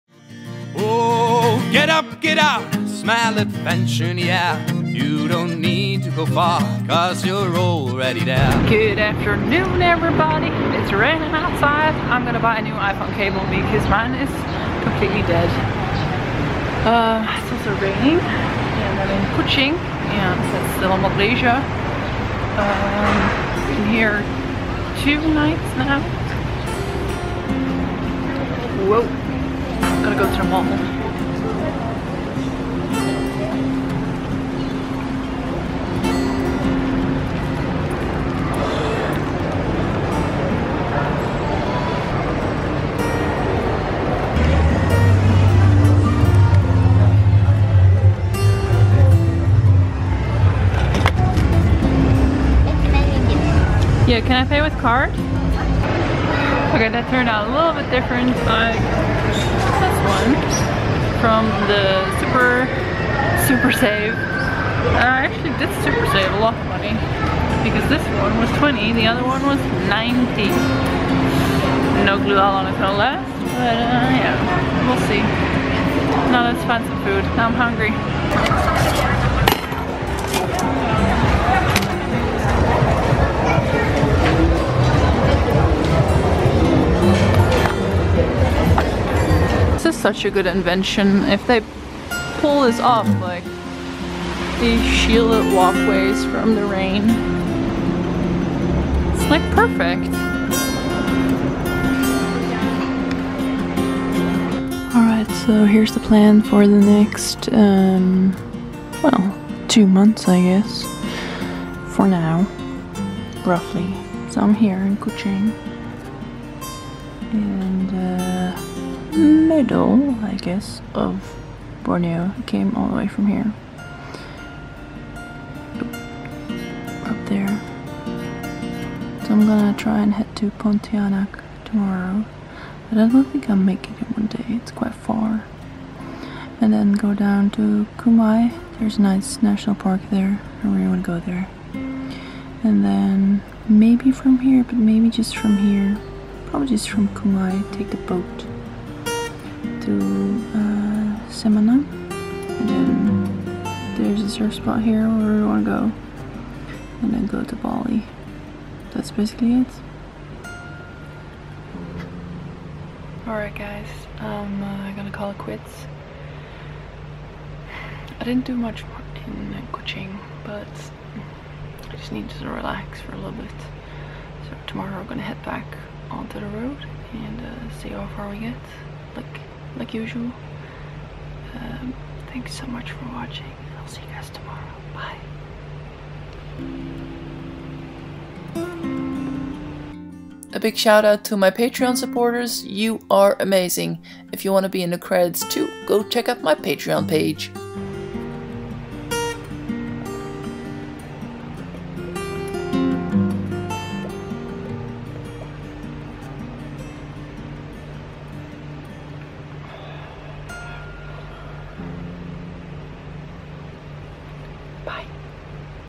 PLANS FOR THE NEXT 2 MONTHS
///DAY 338
After a short trip to the mall I explain what my plans are for the next 2 months.